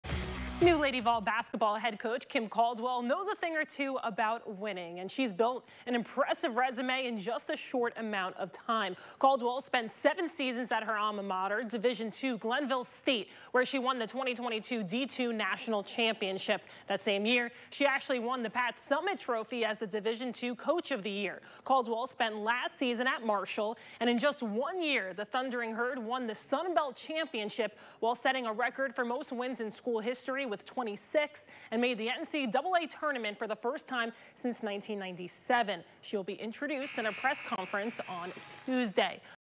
Caldwell brings winning pedigree to Lady Vol program
WATE sports reporter Sam Rothman talks new Tennessee Lady Vols coach Kim Caldwell and looks back on her coaching career.